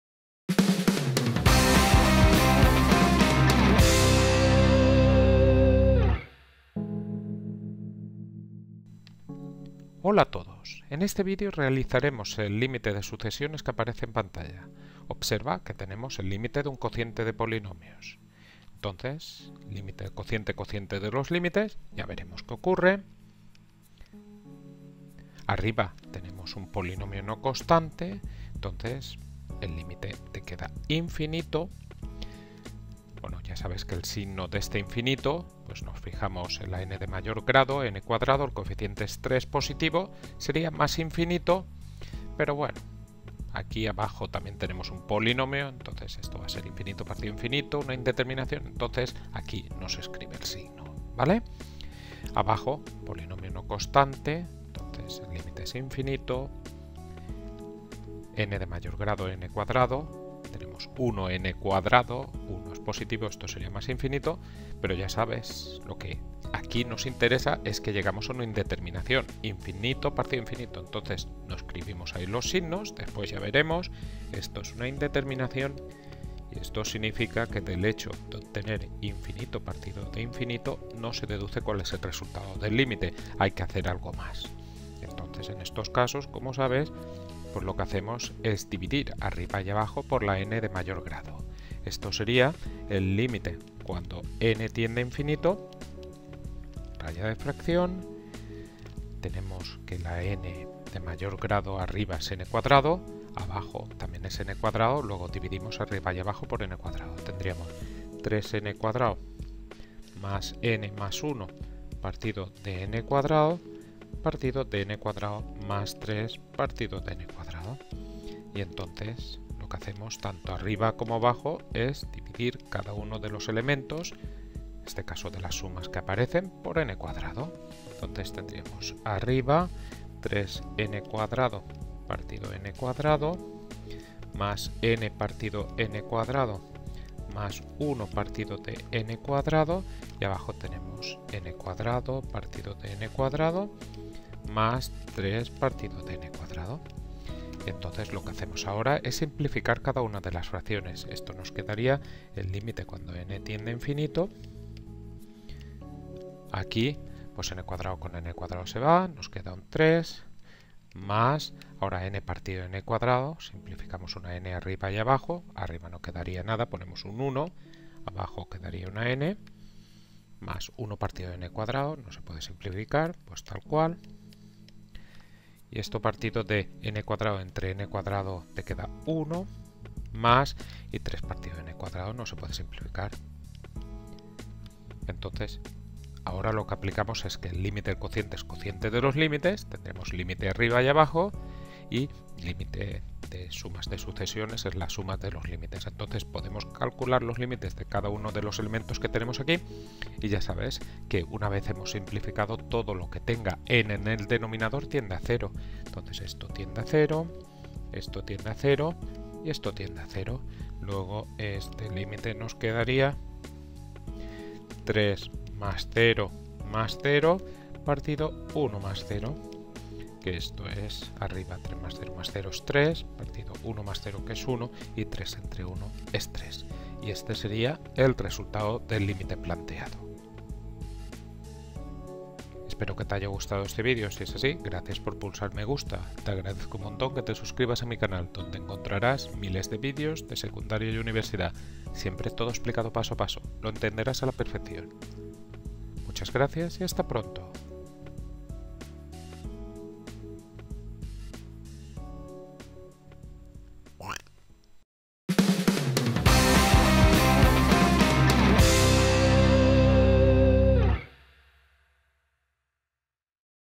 LÍMITES de SUCESIONES, Indeterminación infinito partido de infinito
Calculamos un límite de una sucesión que es un cociento de polinomios correspondiente a la indeterminación infinito partido de infinito.